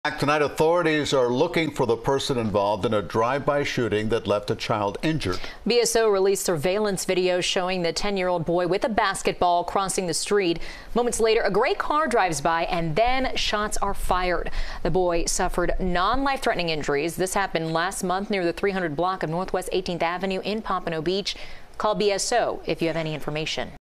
Video shows drive-by shooting of 10-year-old boy in Pompano Beach
The Broward Sheriff's Office has released surveillance video of a drive-by shooting that left a child injured.